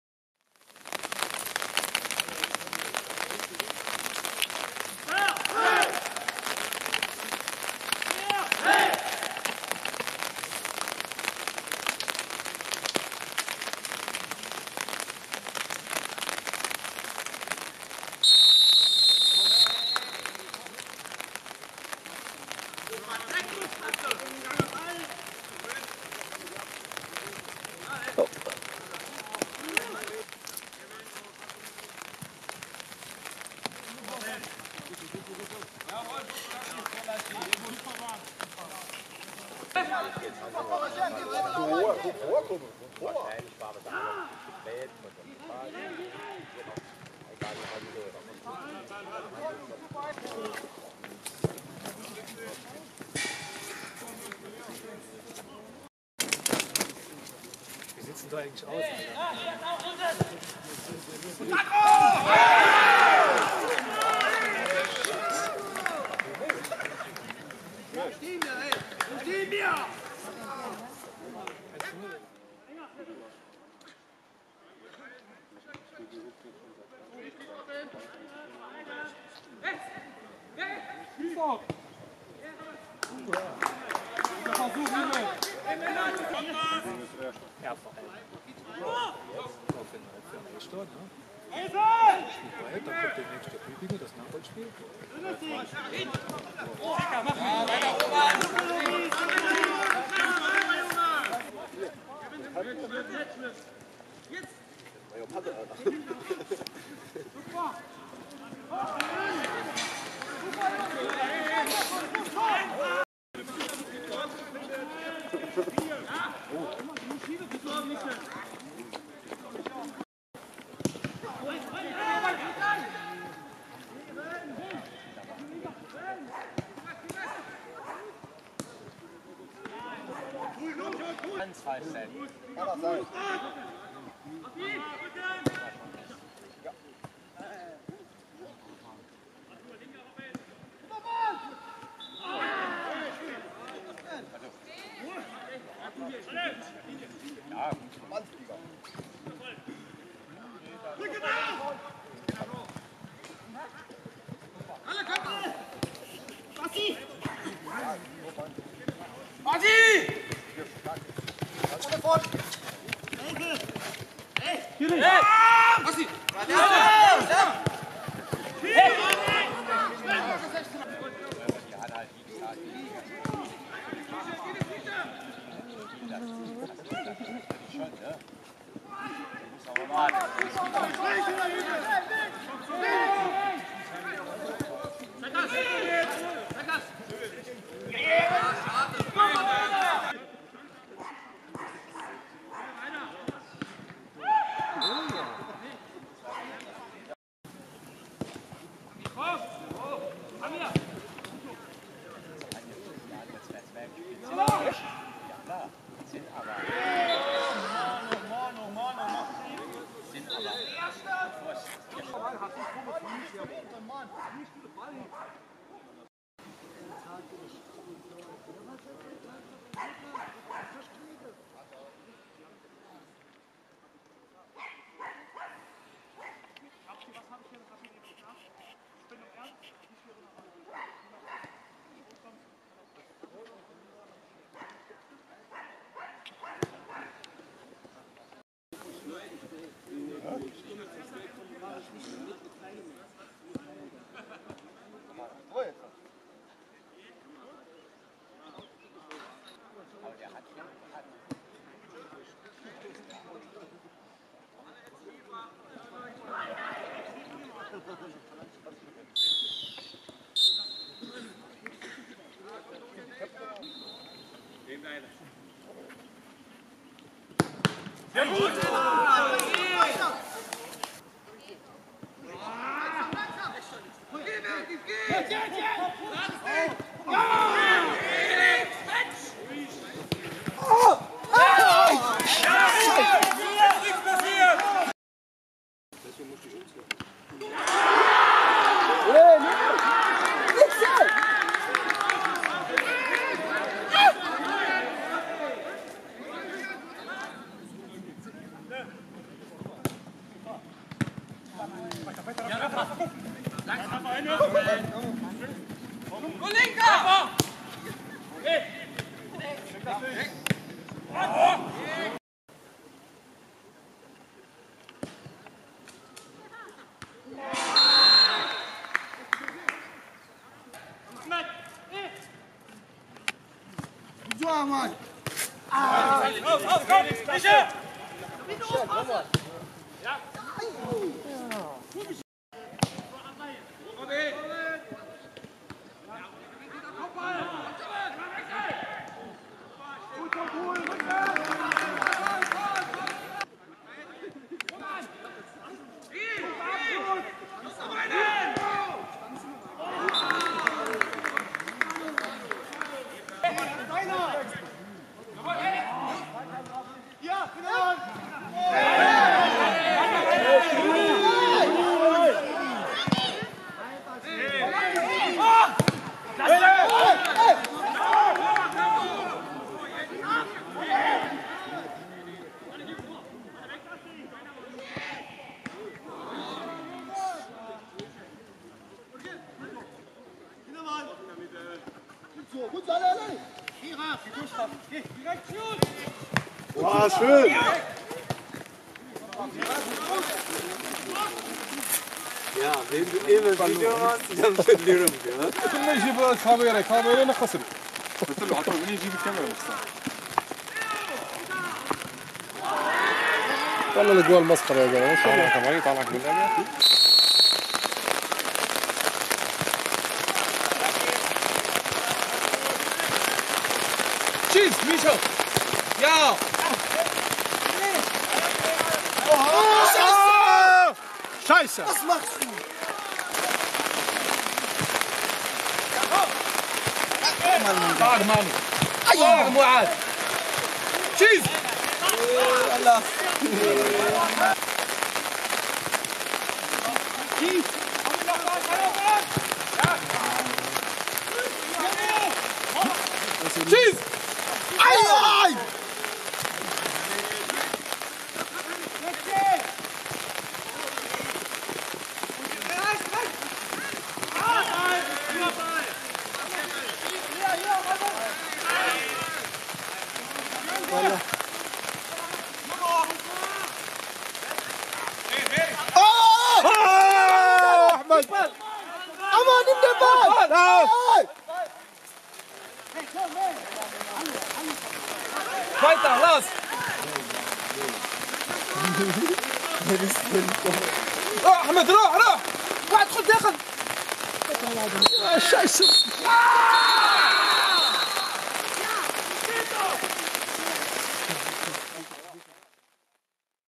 DJK Neuweiler - TuS Jägersfreude – 1:2 (03.11.2019)
Kreisliga A Halberg 2019/20, 14. Spieltag, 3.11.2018 | DJK Neuweiler - TuS Jägersfreude – 1:2 (0:1).

00:56   0:1 Pupo, Alessandro (10.)
04:45   0:2 Schulz, Robin (59.)
07:37   1:2 Al Ktifan, Ahmad (90.)